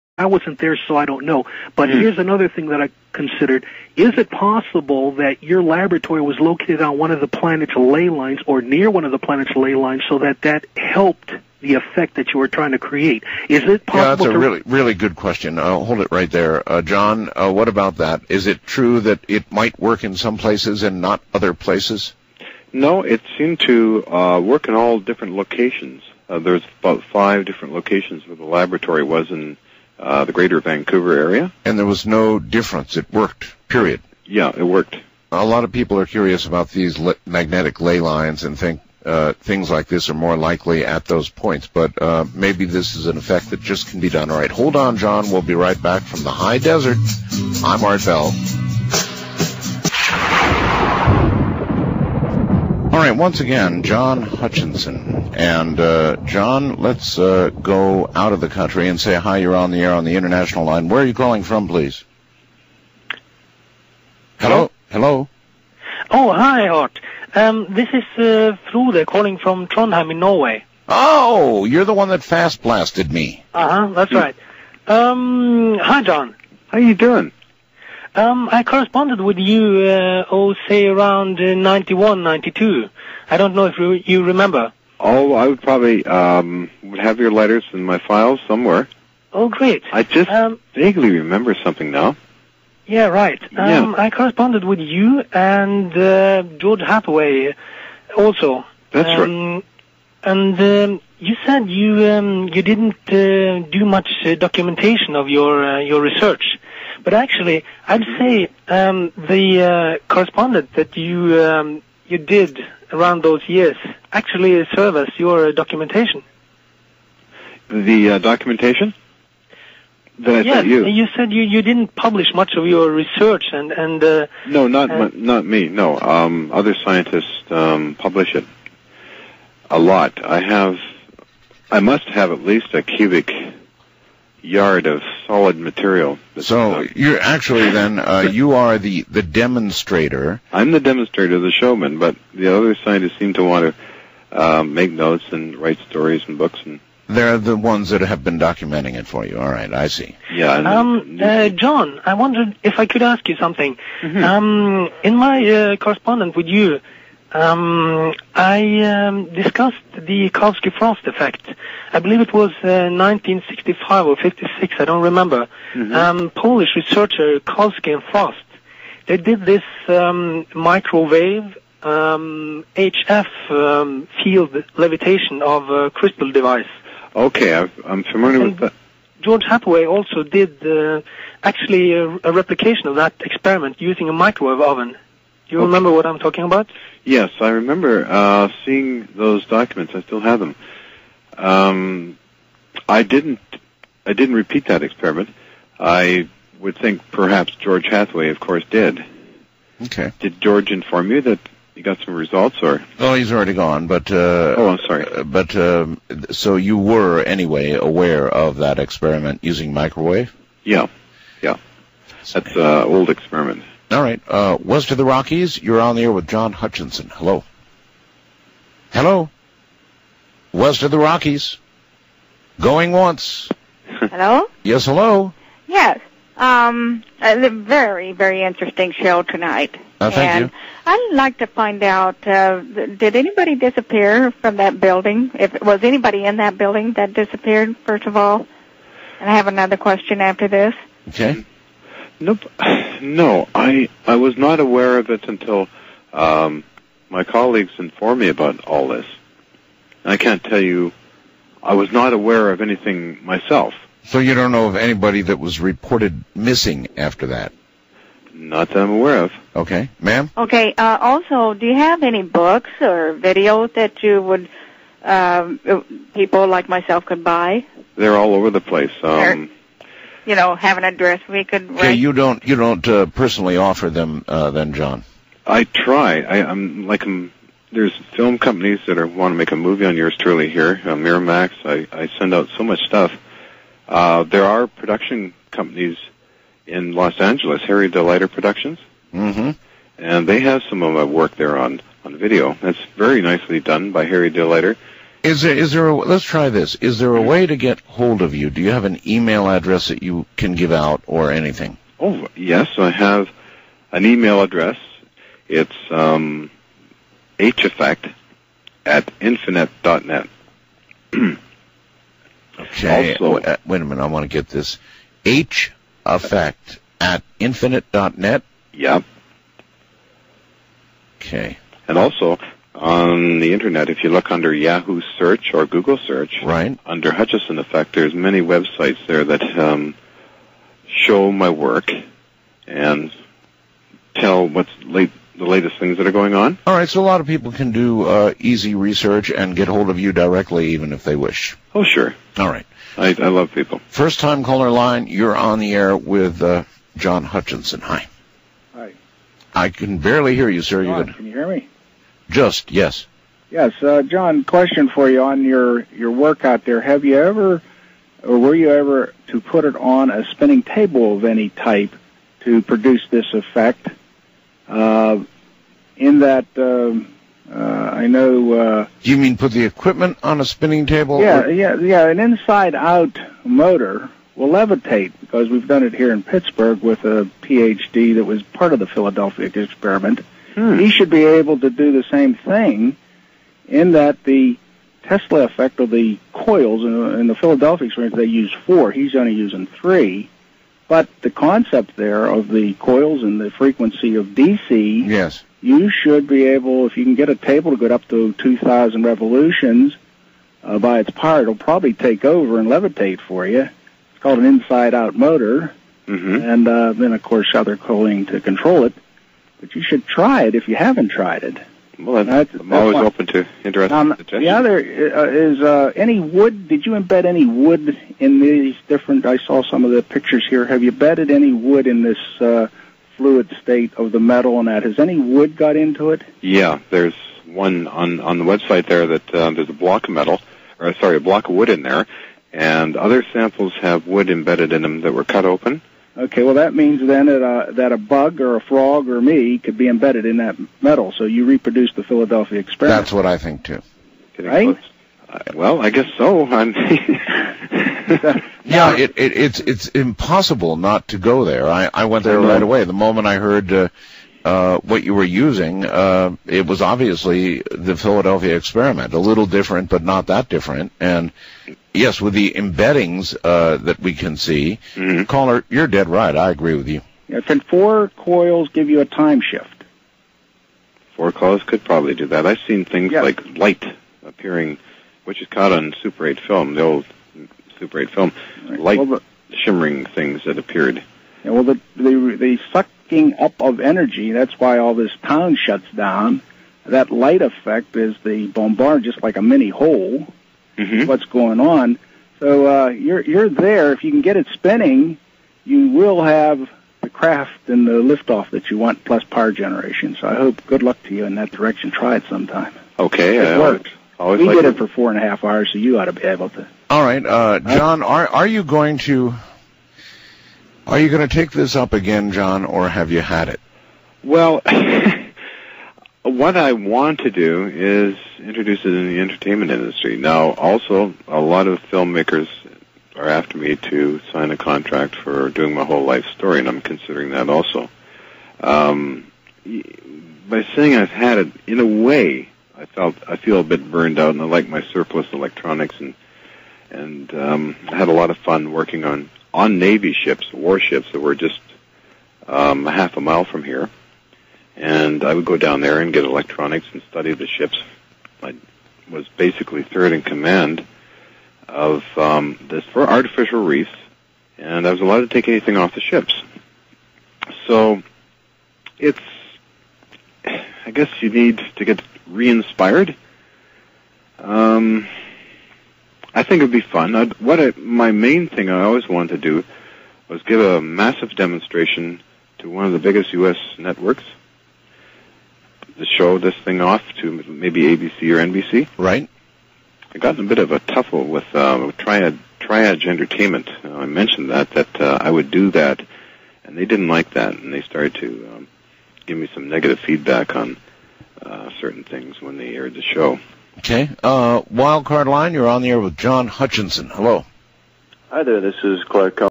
Art Bell - Hutchinson Effect 010
Canadian John Hutchison is a self-taught scientist who reports to have accidentally stumbled onto a collection of phenomena now known as the Hutchison Effect during attempts to study the longitudinal waves of Tesla back in 1979. Some of the effects produced include levitation of heavy objects, metal samples turned transparent, metals turned into jelly, and the spontaneous fracturing of metals. The Hutchison Effect has been well documented both on film and videotape, and has been witnessed many times by numerous credentialed scientists and engineers. His findings have been televised in the U.S., Japan, and Canada. They have also piqued interest from electrical engineering, aircraft companies, the Canadian Department of Defense, and the military.